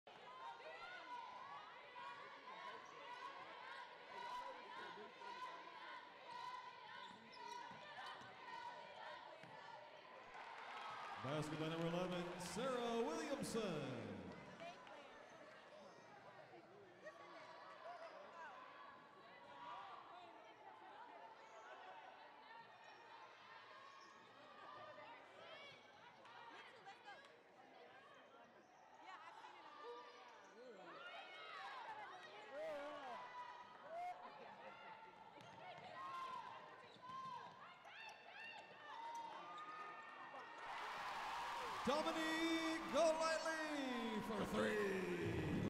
Women's Basketball: ACU vs. Schreiner - November 13, 2017
I was the technical director of this game. I switched using a Panasonic AV-HS6000 switcher.